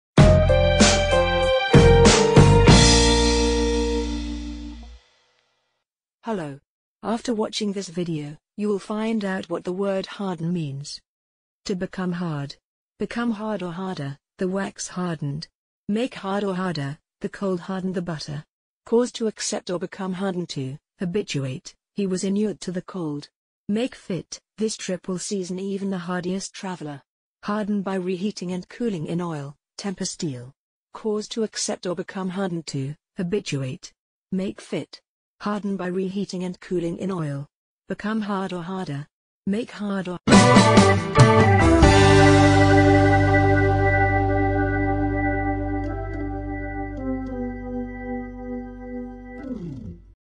Definition of the word "Harden"
you will find full information about this word "harden".
From the video you will learn the definition for the word harden. The lexical meaning of the word harden. What does the word harden mean? And much, much more...